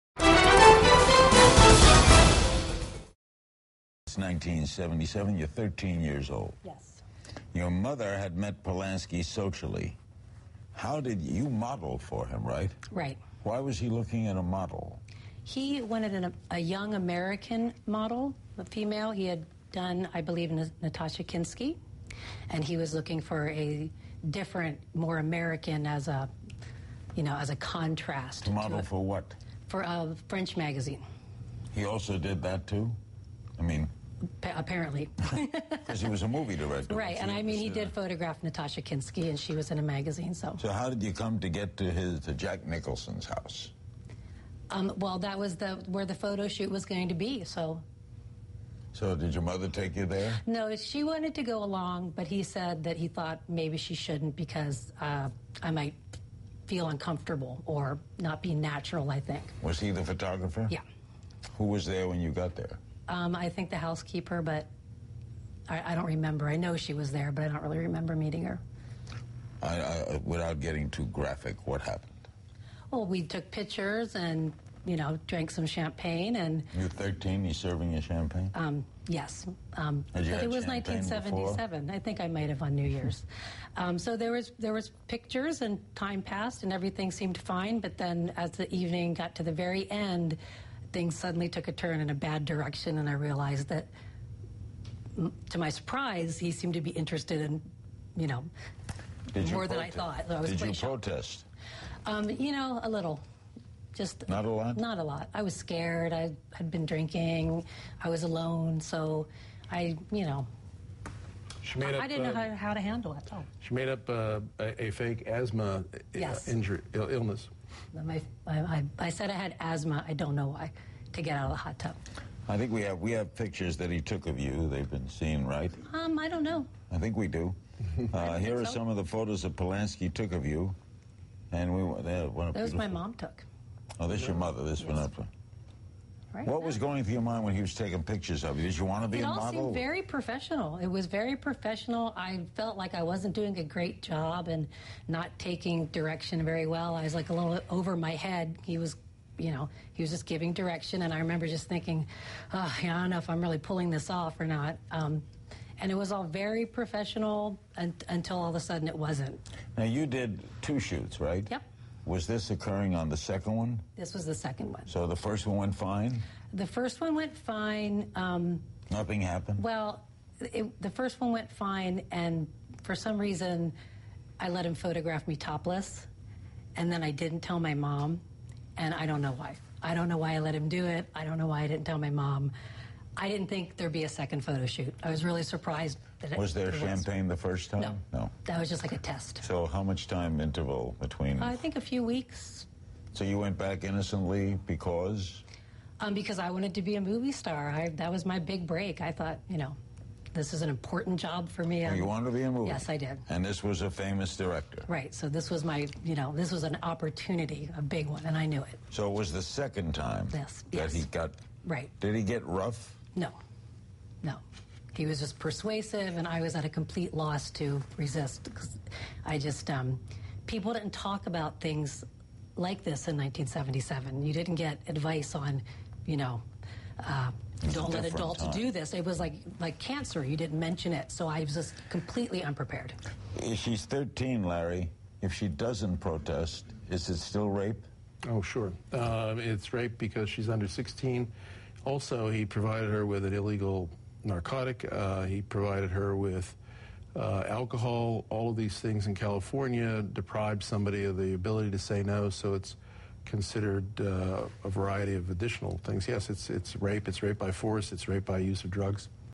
CNN Official Interview: Roman Polanski's victim tells what happened
Samantha Geimer breaks her silence about the incident between her and filmmaker Roman Polanski.